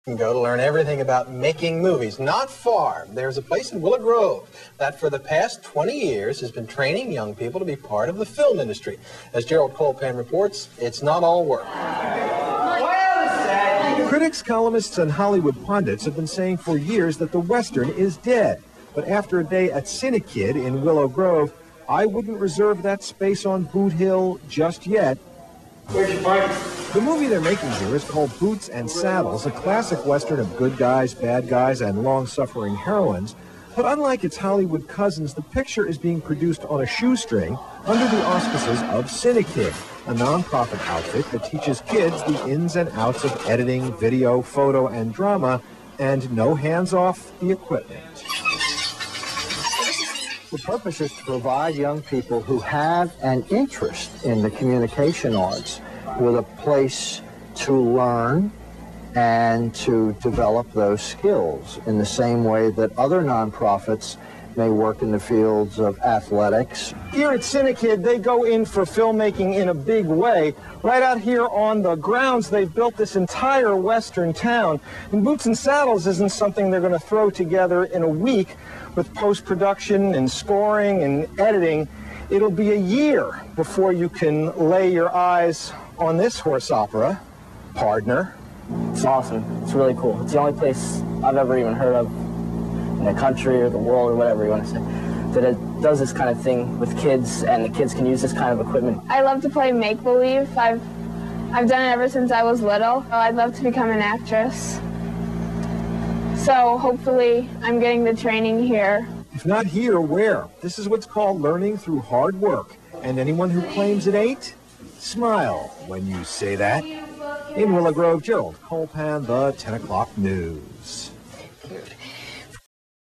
Cinekyd "Boots" News Story
Summer 1995. Behind-the-scenes local news story of the Cinekyd western production "Boots & Saddles." It's "each one - reach one," the organization's motto. Kids and teens make a cowboy adventure.